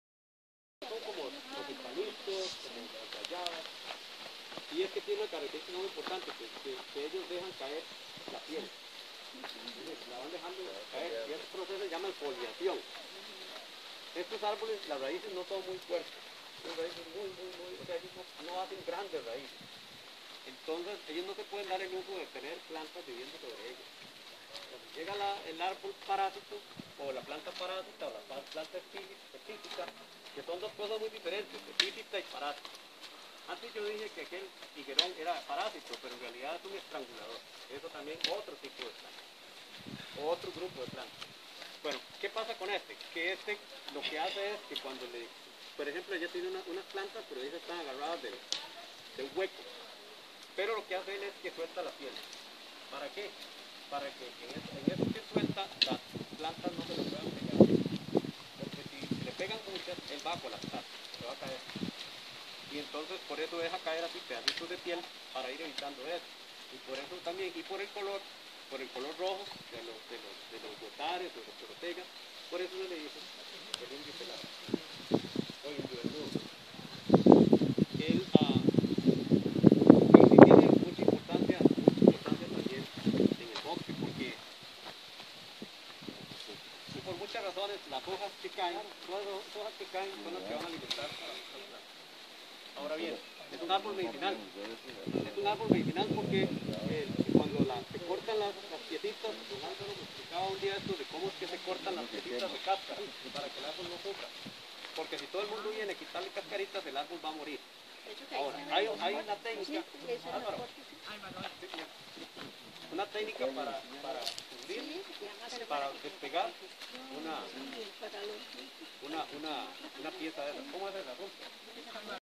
08:30 A. M. Paso por El Ginocuabe. Caminata Palmitos de Naranjo. 30/01/2021. Video #6
Una nueva ilustración de botánica, esta  vez respecto al àrbol de Ginocuabe, o Indio Pelado (Bursera simaruba)